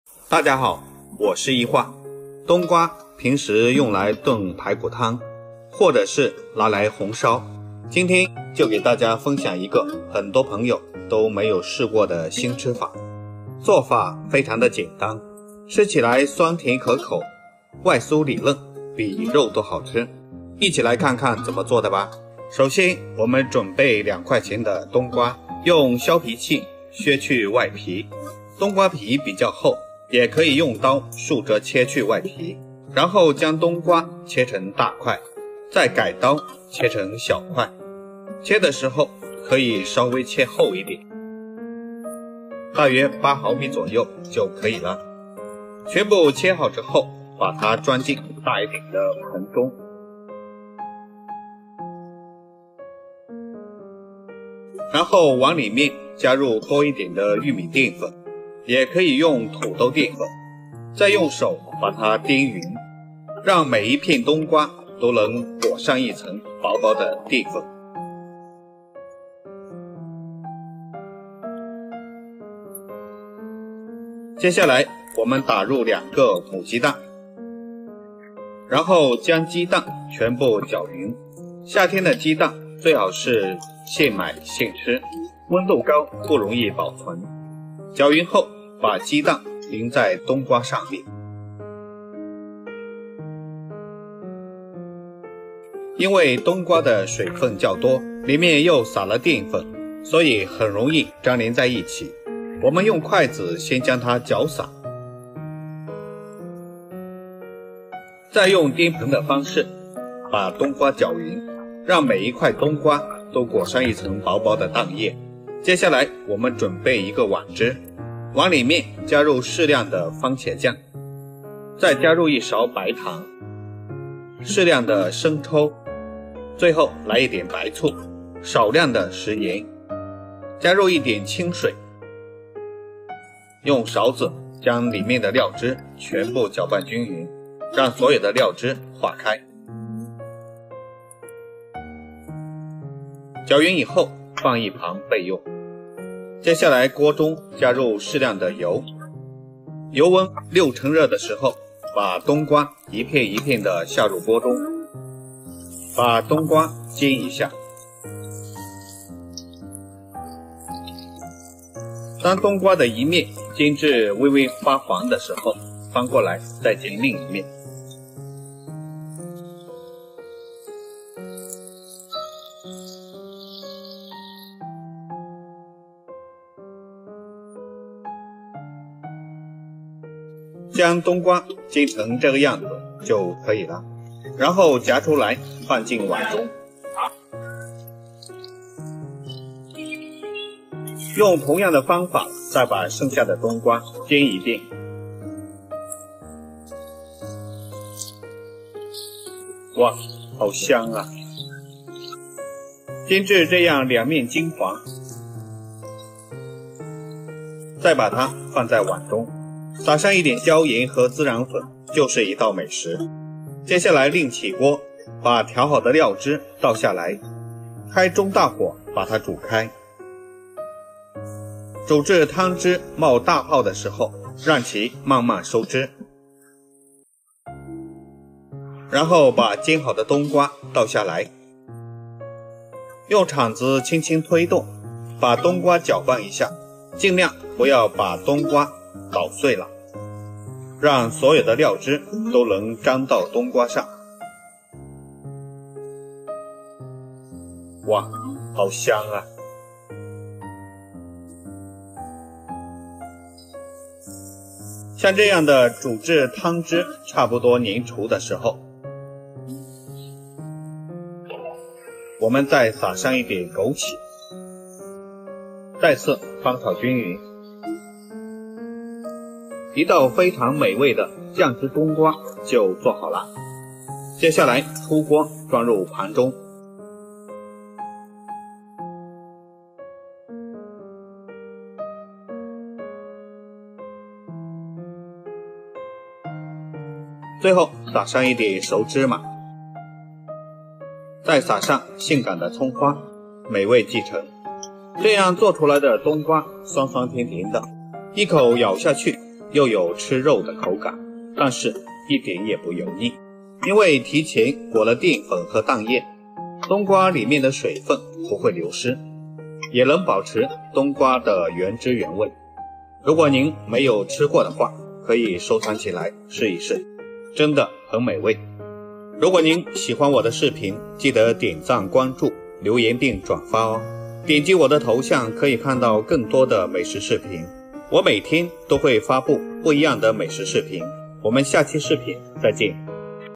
入秋后要多吃冬瓜，头一次见这种做法，淋入两个鸡蛋，比吃肉还香【一画美食】
今天给大家分享的是入秋后要多吃冬瓜，头一次见这种做法，淋入两个鸡蛋，比吃肉还香【一画美食】